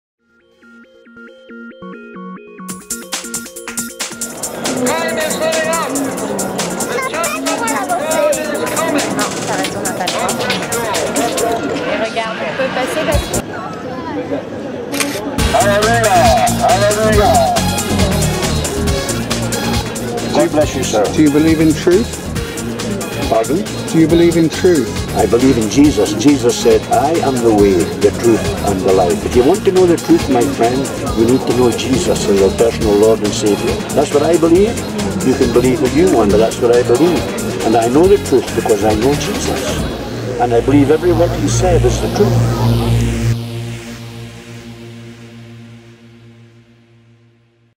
Do You Believe in Truth
Part of a series about 'truth' made with the intention of getting people talking. The series is 'Where is Truth now'. I was grateful to this gentleman for answering questions asked by a stranger.